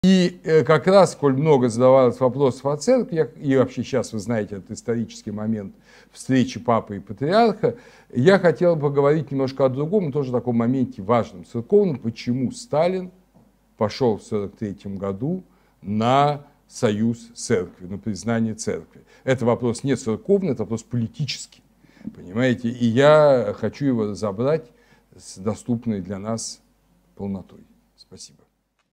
Лекция А.Б. Зубова «Политика коммунистов в отношении Церкви» (АНОНС)
В среду, 16 марта, в 19-00, в рамках кафедры истории «Новой газеты» доктор исторических наук, профессор Андрей Борисович Зубов прочитает лекцию на тему «Как развивалась политика коммунистов в отношении Церкви в последнее десятилетие сталинского режима (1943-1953 гг.)».

В 1942 г. стали заметны некоторые послабления и изменения в политике коммунистического режима СССР в отношении религии и, особенно, Русской Православной Церкви. Во время встречи 4 сентября 1943 г. трех митрополитов со Сталиным в Кремле эти изменения получили четкое оформление. Можно ли считать, что Сталин изменил свое отношение к вере и церкви? Или эти изменения были результатом как вынужденных обстоятельств, так и расчетов коммунистов использовать Церковь в своих целях, которые в сравнении с 1930-ми годами не претерпели существенных изменений. Об этом в лекции 16 марта 2016 г. в 19:00.

Чтобы лично присутствовать на лекции и иметь возможность задать свои вопросы докладчику или заведующему кафедрой, приходите в редакцию (Потаповский переулок, 3). Если вы хотите получать почтовую рассылку с анонсом лекций «Новой газеты», отправляйте на эту же почту письмо с темой «хочу в рассылку».